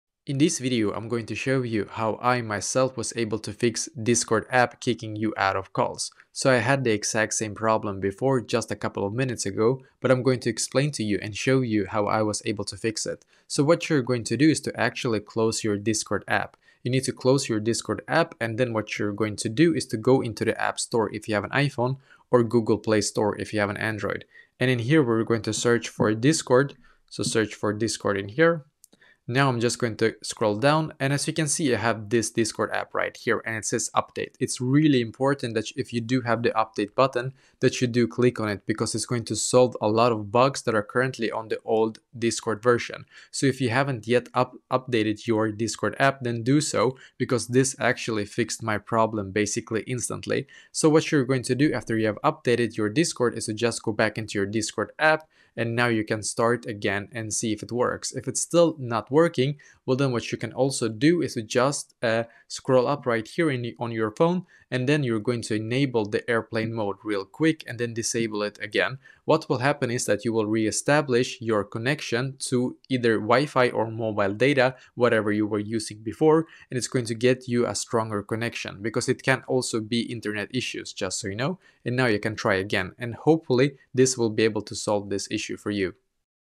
How To Fix Discord App Kicking You Out Of Calls (EASY 2025)
If Discord keeps kicking you out of calls, follow this guide to troubleshoot the issue and enjoy uninterrupted communication.